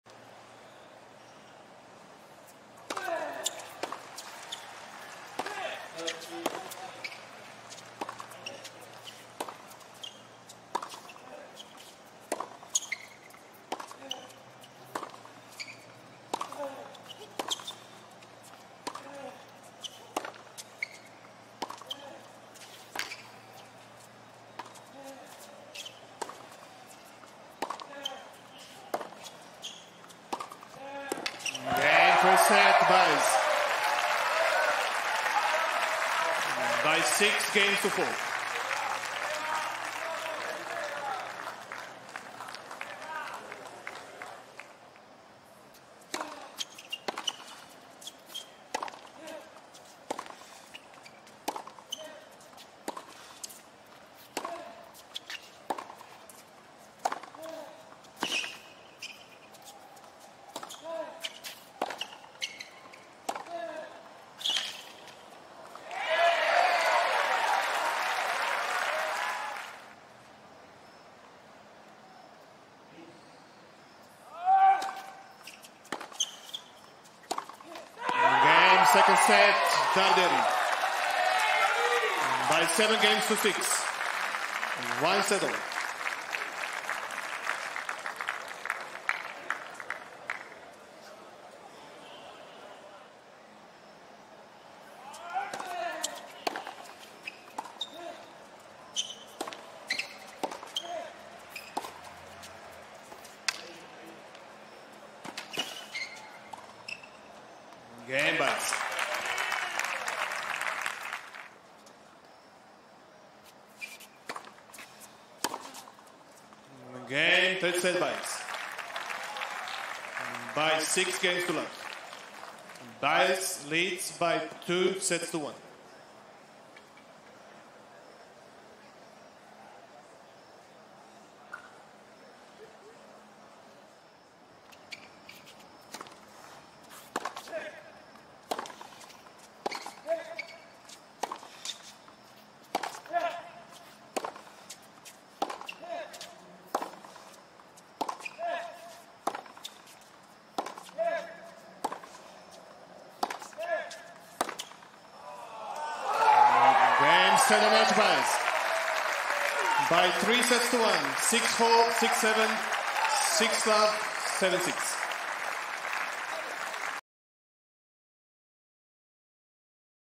Sebastian Baez vs. Luciano Darderi Highlights | 2024 US Open Round 1
Watch the highlights of Sebastian Baez vs. Luciano Darderi in Round 1 of the 2024 US Open. These highlights are presented by Cadillac.